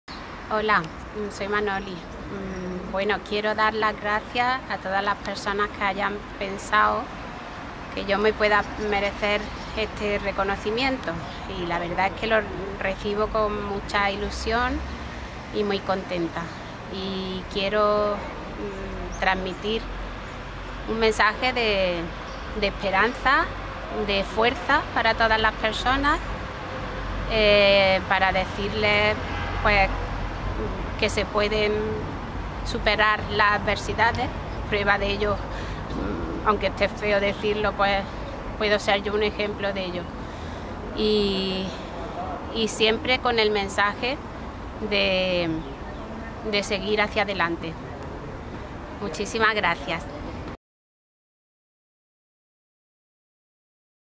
💚🎥🏅 Manoli González - Efebo 2022
👉 Inclusión para Manoli González Aguilar que agradece la distinción y lo recibe con mucha ilusión.